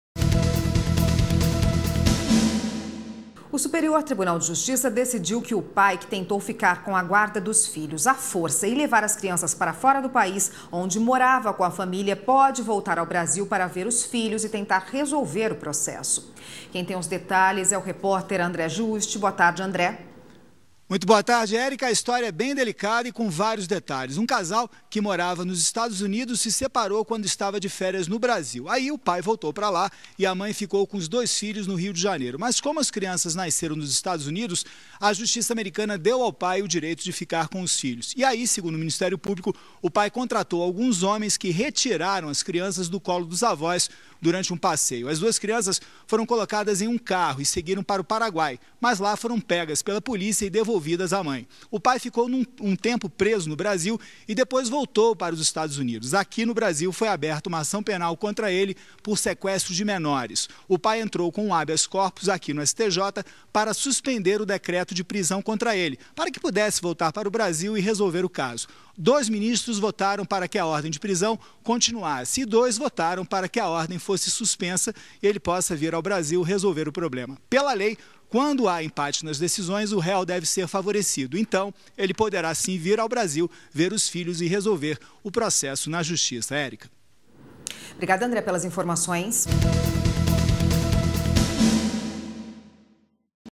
Jornal da Justiça- Guarda dos Filhos
O Superior Tribunal de Justiça (STJ) decidiu que o pai que tentou ficar com a guarda dos filhos a força e levar as crianças para fora do país onde morava com a família, pode voltar ao Brasil para ver os filhos e tentar resolver o processo.

Veja os detalhes na reportagem de André Giusti!